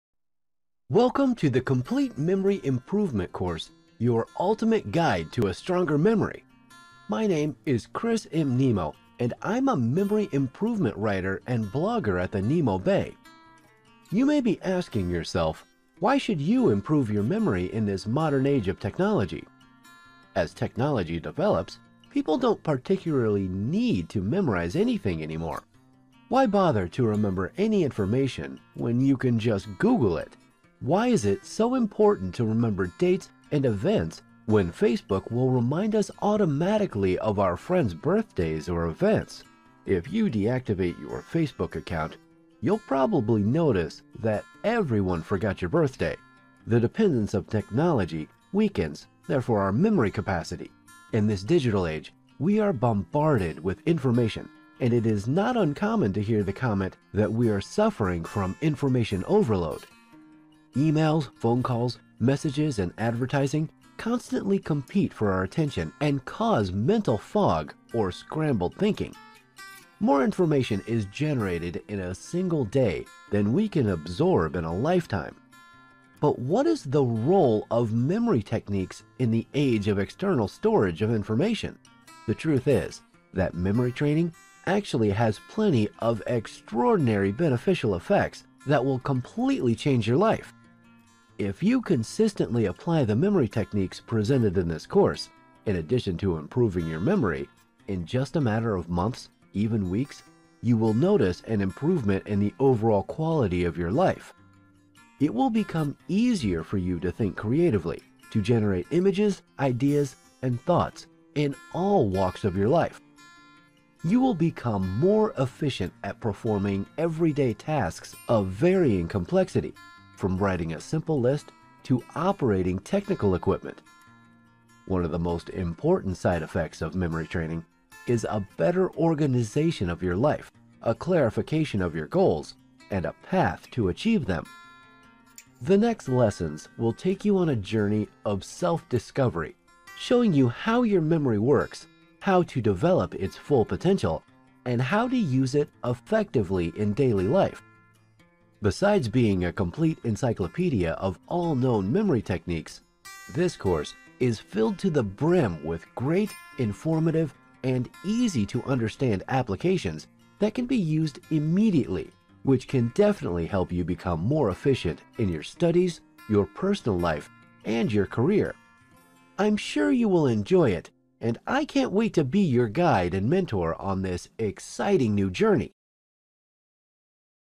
The Complete Memory Improvement Course: Your Ultimate Guide to a Stronger Memory
I can confidently say that this is the BEST and the MOST COMPLETE memory improvement course I’ve ever released!
 What is the challenge of this course?
   In the first place, I want to make you understand what memory improvement is about, and see for yourself just how simple and handy this skill is!
 After you understand how it works, and I guarantee that you'll understand it after watching this course, improving your memory will be just a matter of consistency and practice.
This course is the result of studying: dozens of books, totaling some thousands of pages, dozens of hours of e-learning courses, and of hundreds of hours of practical exercises and I think that it can give you a clear picture of what this wonderful hobby and skill can do for your life.
In these lessons, you will find actionable, easy-to-apply concepts and activities that can be used immediately.
Keep in mind that these methods and systems are tested and used by memory experts and they will also work for you!
 If you consistently apply the memory techniques presented in this  course, in addition to improving memory, you will notice an improvement in the overall quality of your life!
 This course will take you on a journey of self-discovery, showing how your memory works, how to develop its full potential, and how to use it effectively in daily life.
 I guarantee, this is the most complete and up-to-date memory improvement course available ANYWHERE on the market—or your money back.

 I'm sure you will enjoy it and I can't wait to be your guide and mentor on this exciting new journey!

This course is an investment in your future!

Chris M Nemo
Memory Improvement Writer and Blogger at The Mnemo Bay